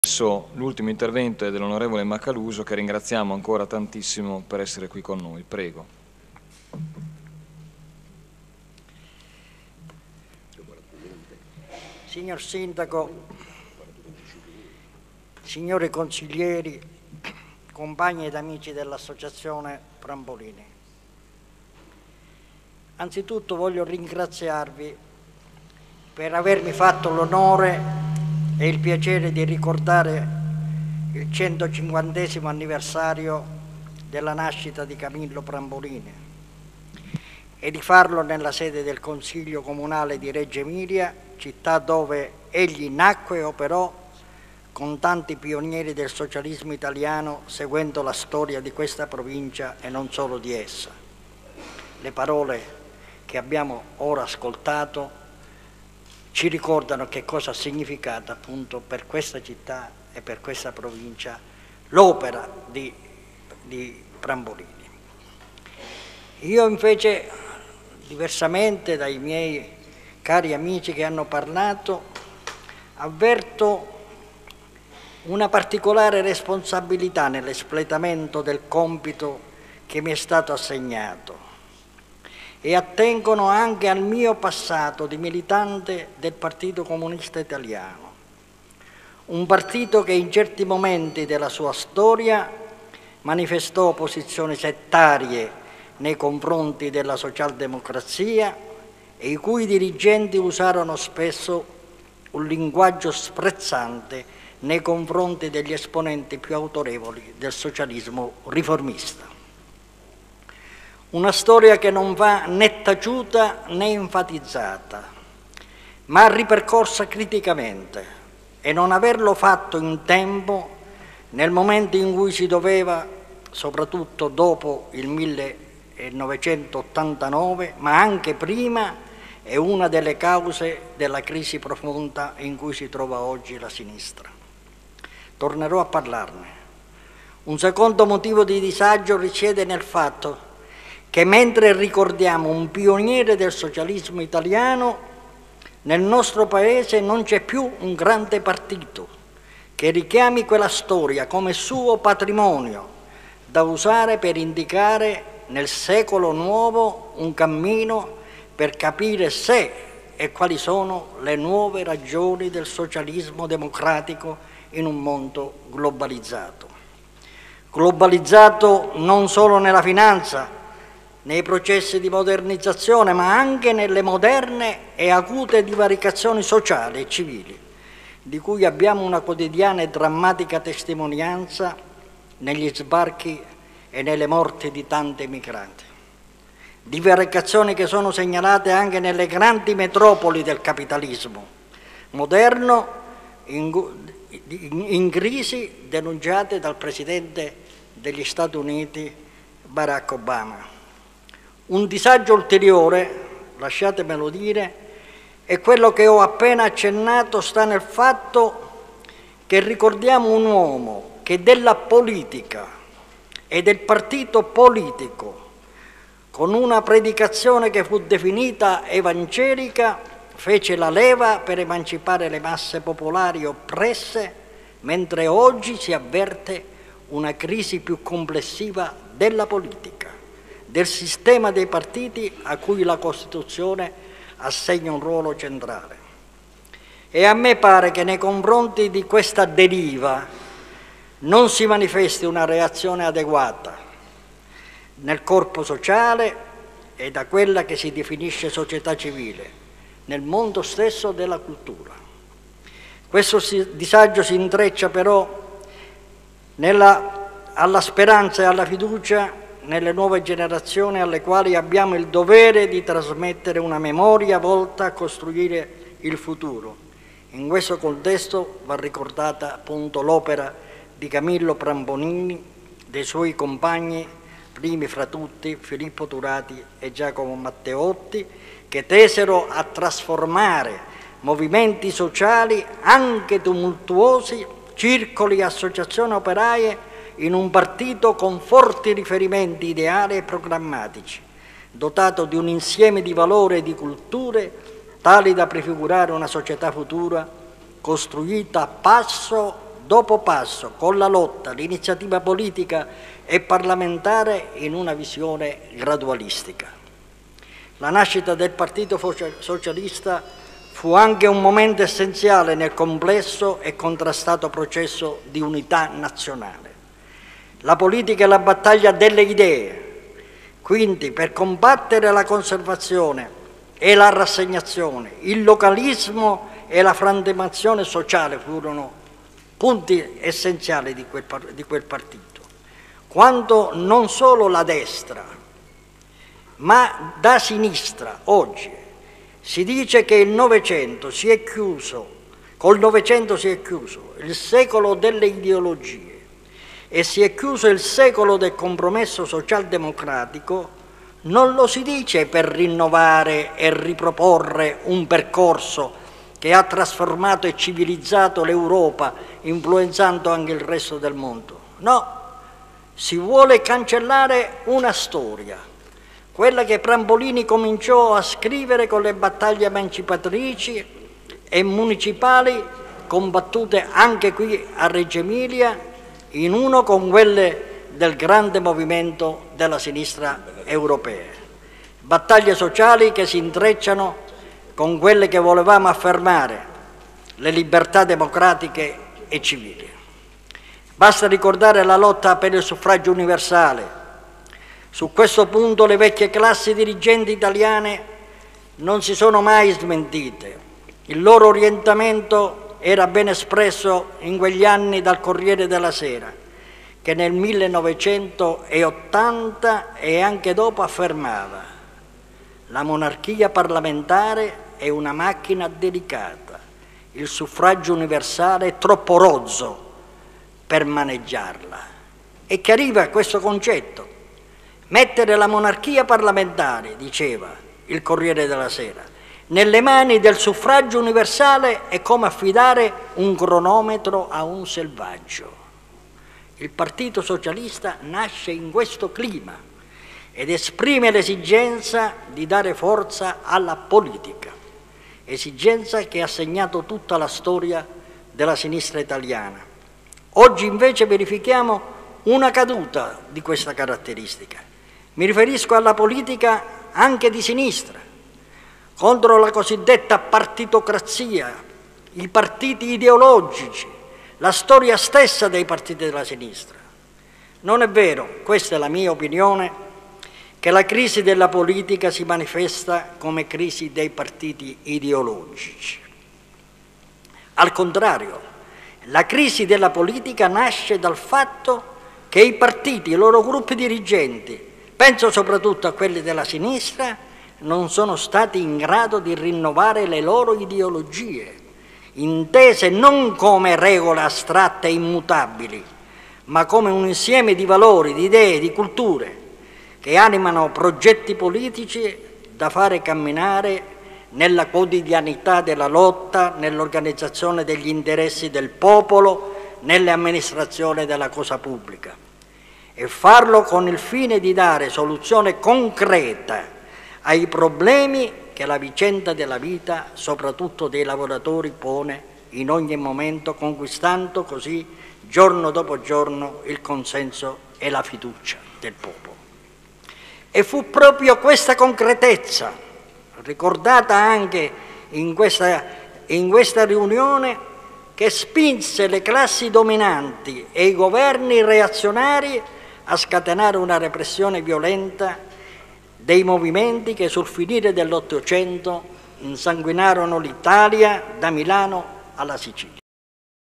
150 anni di riformismo - Intervento dell'On. Emanuele Macaluso (parte 1)
150 anni di riformismo - Intervento dell'On. Emanuele Macaluso (parte 1)